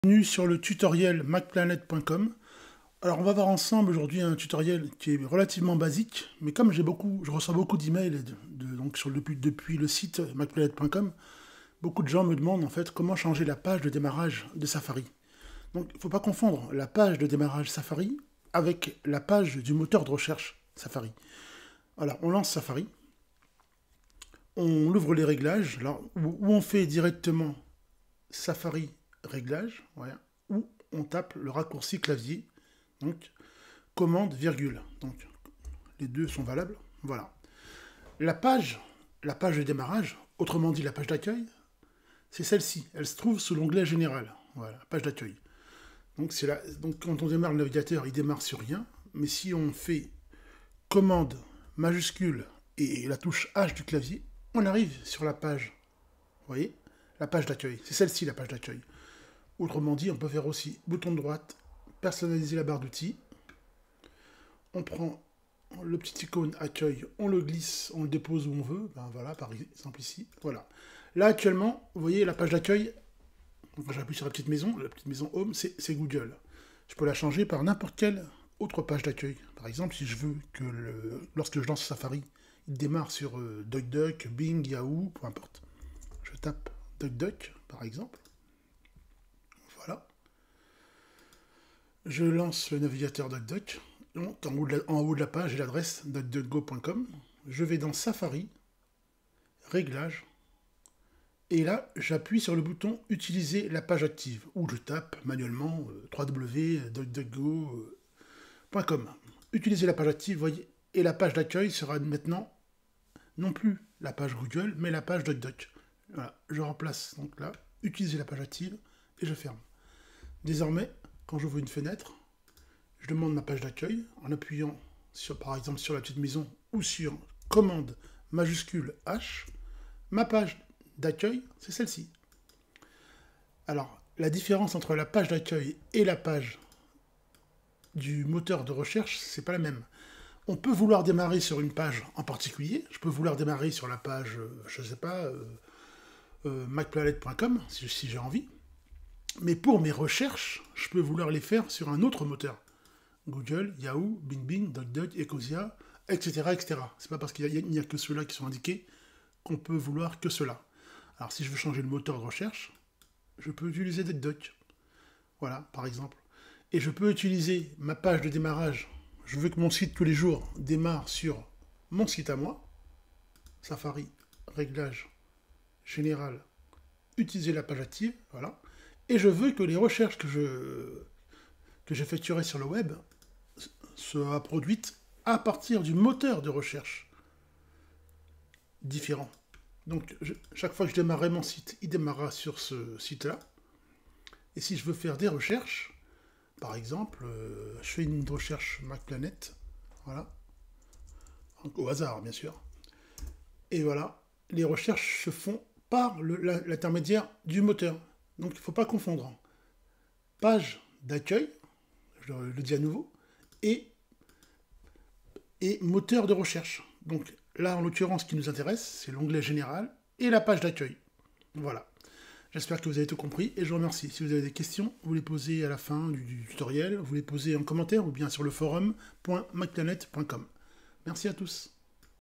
Comment changer la page d'accueil de Safari sur Mac ?
Comment changer la page d'accueil de Safari Mac ? Autrement dit la page de démarrage par une page de votre choix. À ne pas confondre avec la page du moteur de recherche qui peut également être différente (DuckDuckGo, Google,  Yahoo, Bing, Ecosia).